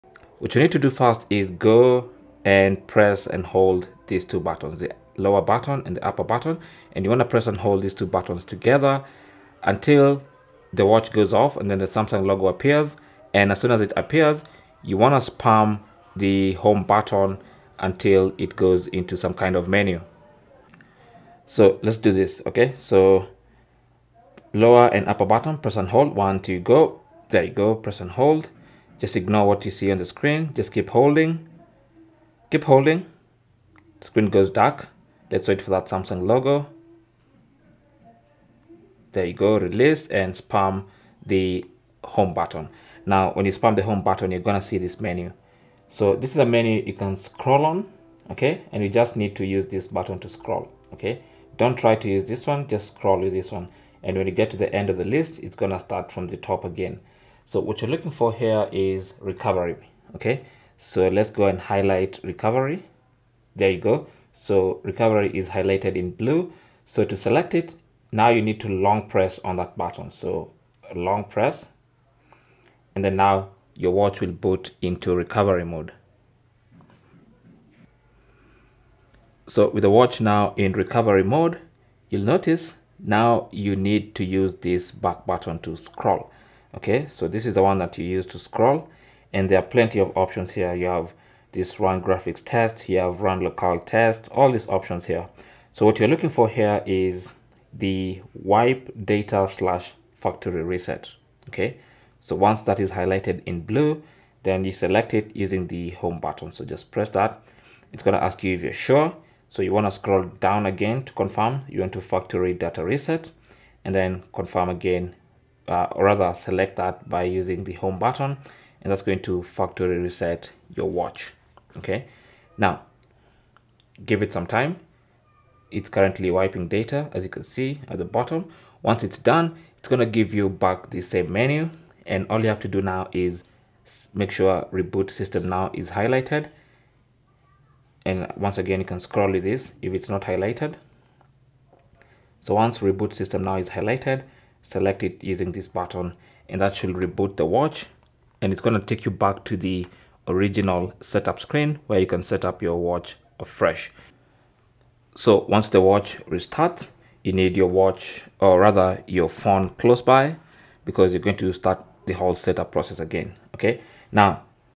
How To Hard Reset Samsung Galaxy Watch 8
Want to learn how to hard reset your Samsung Galaxy Watch 8?

In this quick and easy tutorial, I’ll show you the step-by-step process to perform a factory reset or hard reset on your Galaxy Watch 8. Whether your watch is frozen, running slow, or you just want to erase all data before selling it, this guide will help you reset it safely.

How To Hard Reset Galaxy Watch 8 Classic

Press and hold the two action buttons on the watch until you see the Samsung logo
Spam the Home button for a few seconds to access Recovery menu
Use the Home button and highlight Recovery
Long-press the home button to proceed
Use the back button to scroll down and highlight Wipe Data/Factory Reset
Select the option using the Home button
Confirm the process
Highlight Reboot System Now
Select it using Home Button
Watch will reboot into the setup screen

Samsung Galaxy Watch 8 /  8 Classic:

Cell Phone Tripod Adapter